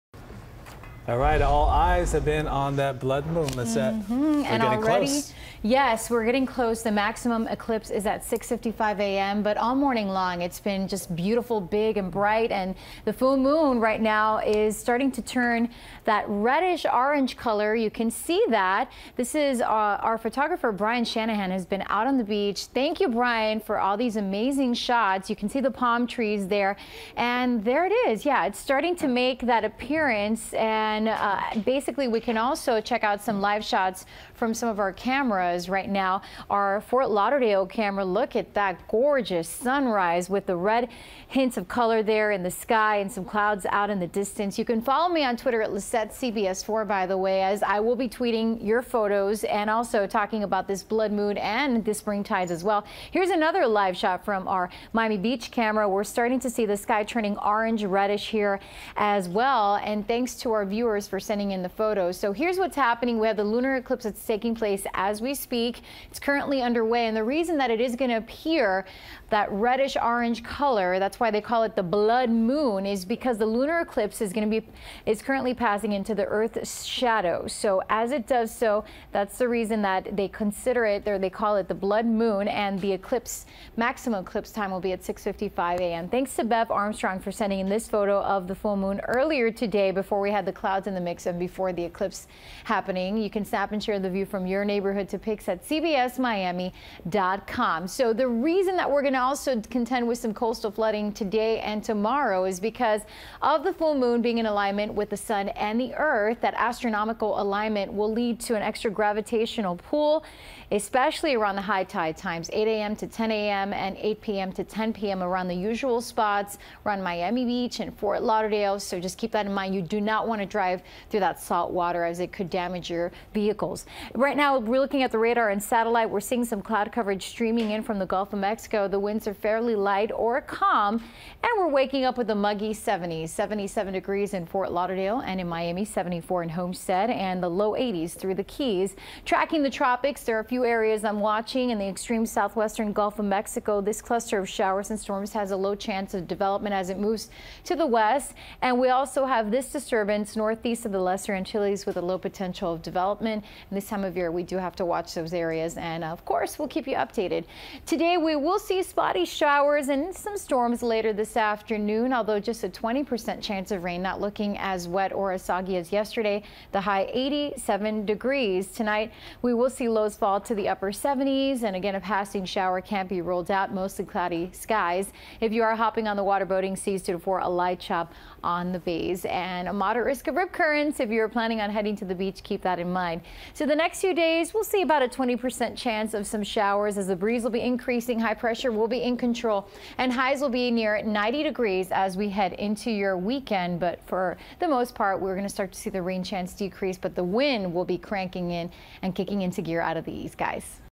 CBSMiami.com Weather 10/8/2014 Wednesday 6AM
Partly sunny. A slight chance of showers this afternoon. Highs in the upper 80s. Northeast winds 5 to 10 mph. Chance of rain 20 percent.